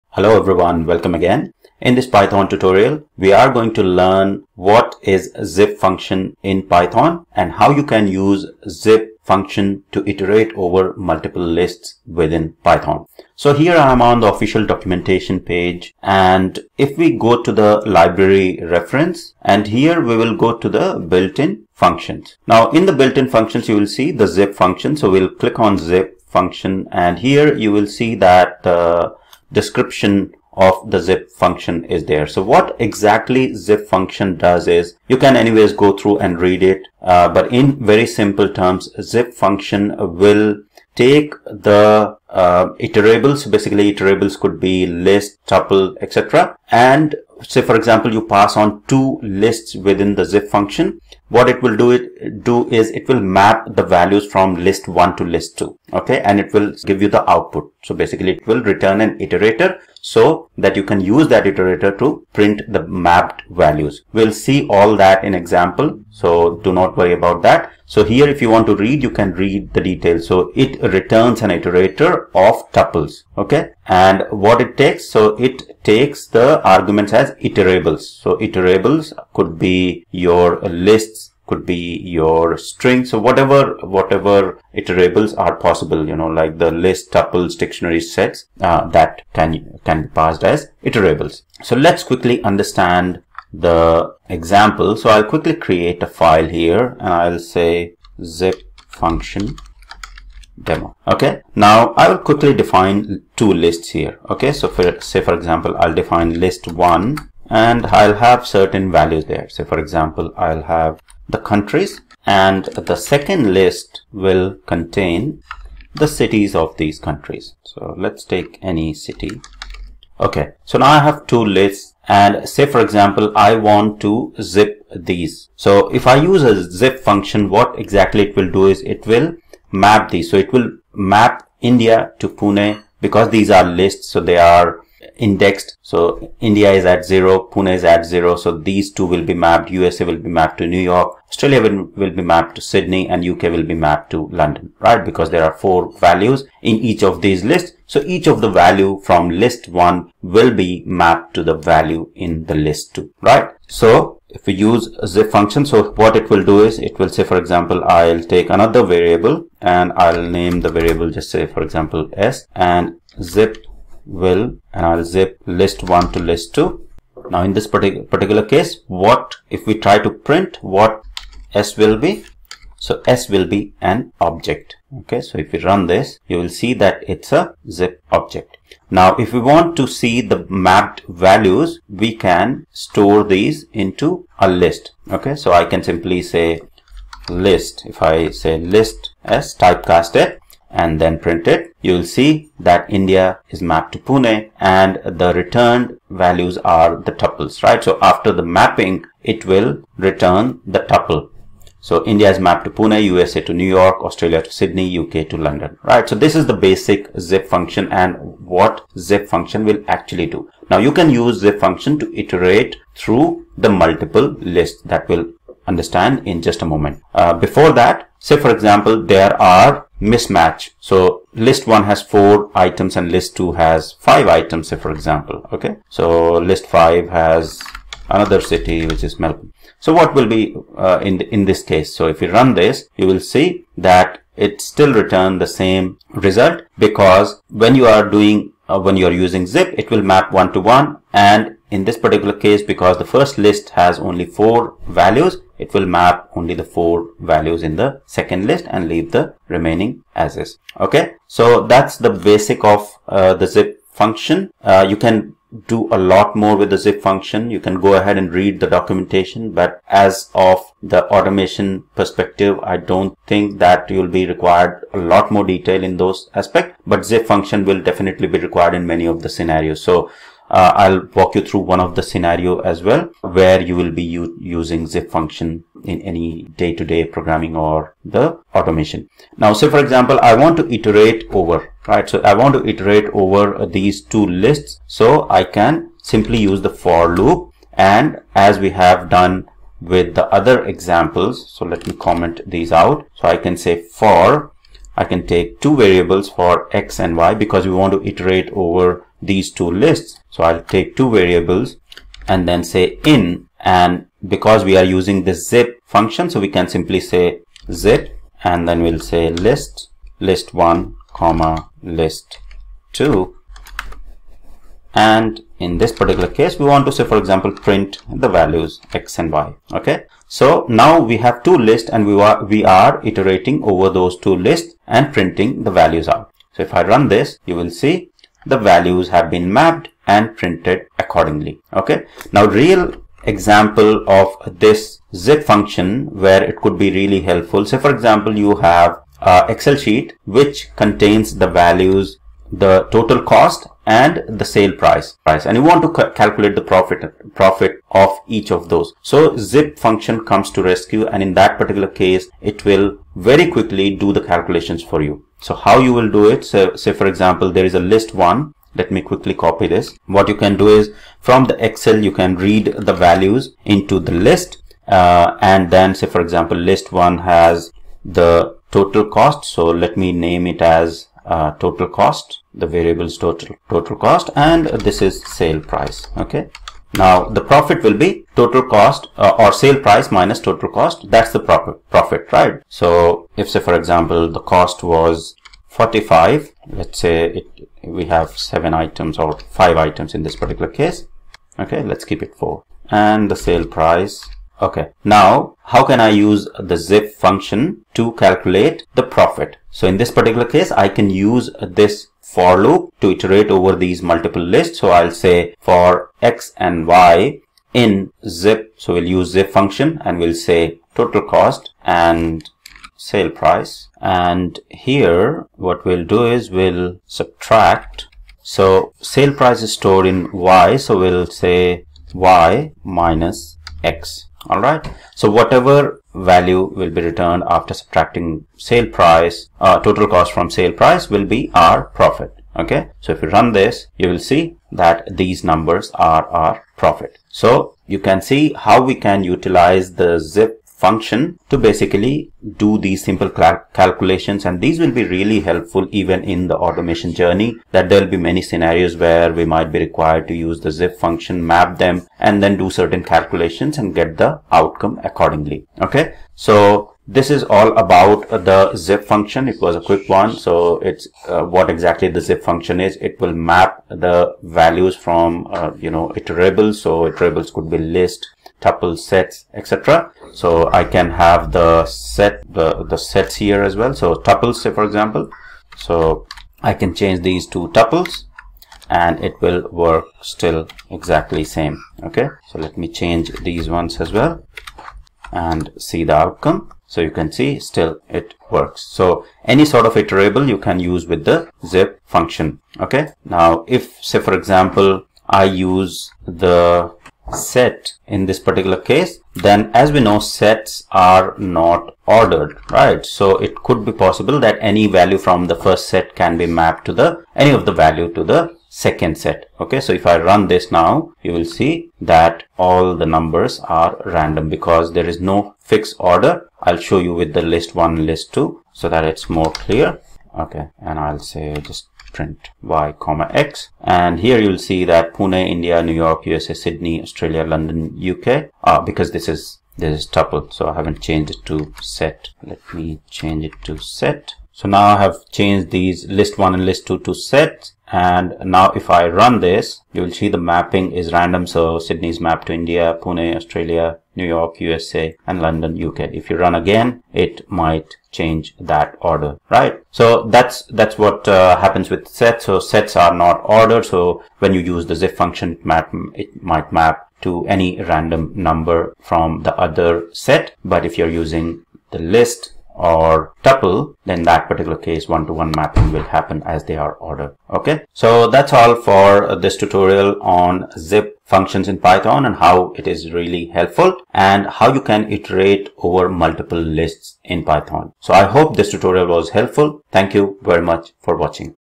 Python for Testers #24 - Zip Function in Python | Zip Function Explained
In this Python for Testers Tutorial we will learn how to use zip function in python. Python Zip function makes an iterator that aggregates elements from each of the iterables. Iterables could be list, tuples, strings.

Zip function returns an iterator of tuples, where the i-th tuple contains the i-th element from each of the argument sequences or iterables.